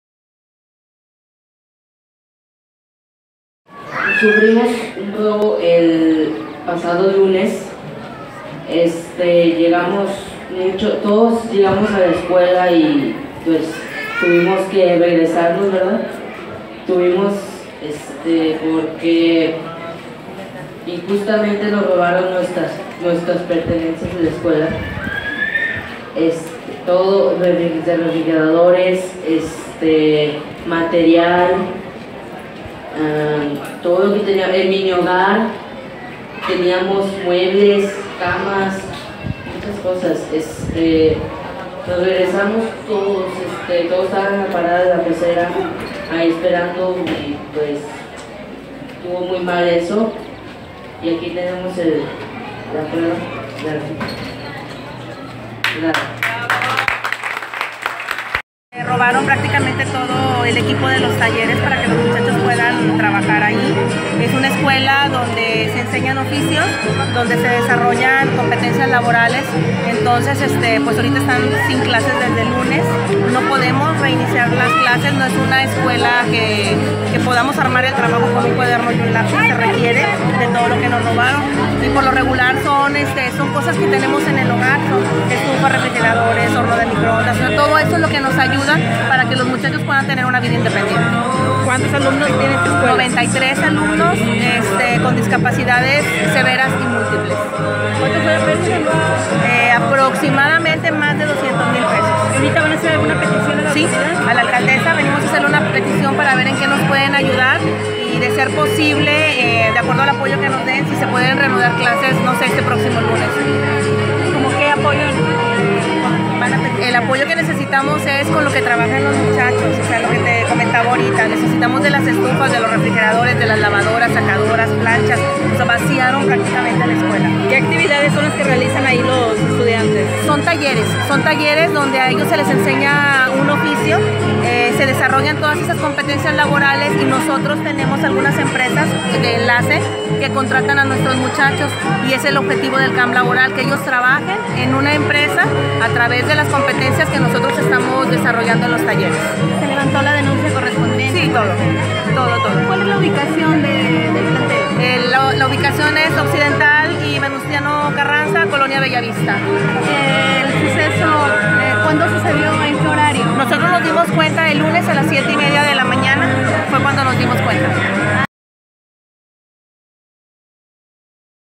Escuela especial pide apoyo a autoridades de Reynosa tras robo
Reynosa, Tamaulipas.- Este pasado fin de semana el Centro de Atención Múltiple (CAM) de Reynosa fue víctima de robo, por esa razón, Anabel Mayela de la Cruz Arredondo, supervisora de la zona escolar 8 de educación especial, acudió con la primer autoridad municipal para solicitar su apoyo para la adquisición de lo hurtado.